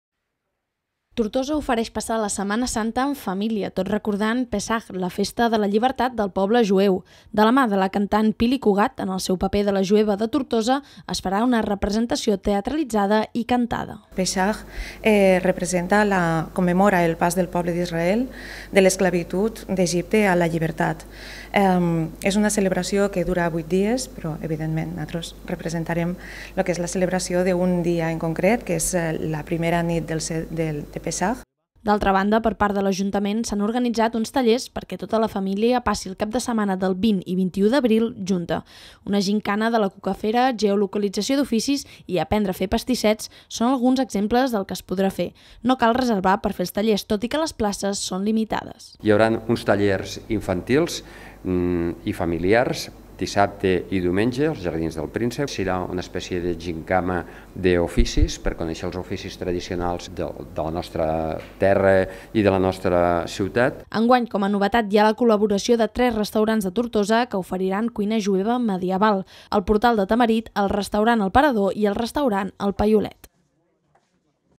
Activitats en família per celebrar la Setmana Santa a Tortosa
A partir de la festa jueva de la llibertat, Tortosa ha preparat un seguit d’activitats de cara la setmana santa per a xalar en família. Tallers de cuina i gimcanes són alguns exemples. Com a novetat, enguany diversos restaurants oferiran menús jueus.